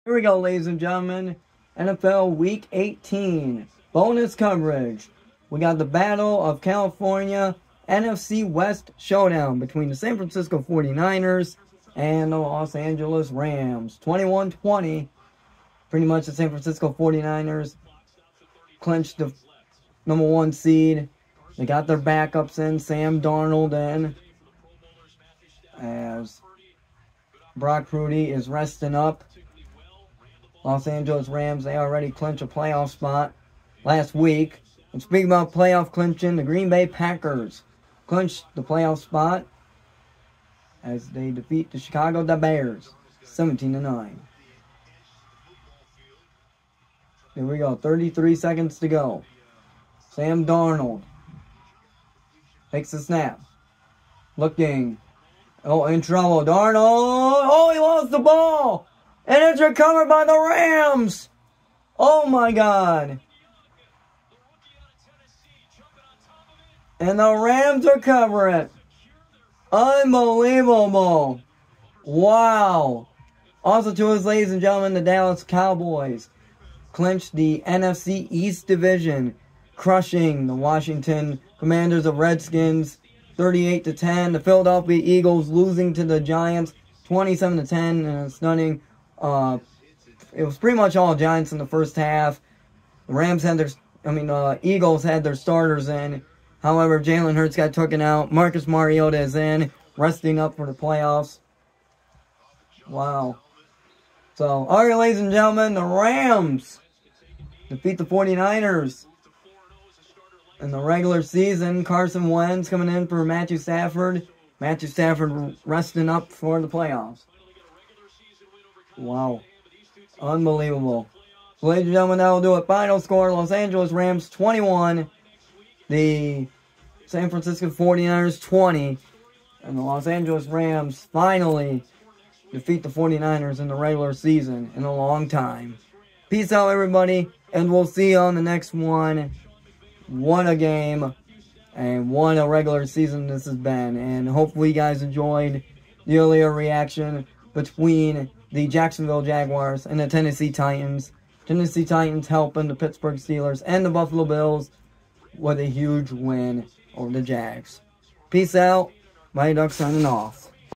NFL Bonus Coverage LA Rams D strip sack Sam Darnold to defeat NFC West rival San Francisco 49ers
To wrap up the 2023-24 NFL regular season bonus coverage of an NFC West rivalry Showdown as the Los Angeles Rams took on the San Francisco 49ers as both teams rested their starters for the playoffs. Sam Darnold was trying to make a game winning for the San Francisco 49ers down by 1 however the Los Angeles Rams defense stepped up big as they stripped the ball away from Sam Darnold to hang on to a hard fought 21-20 victory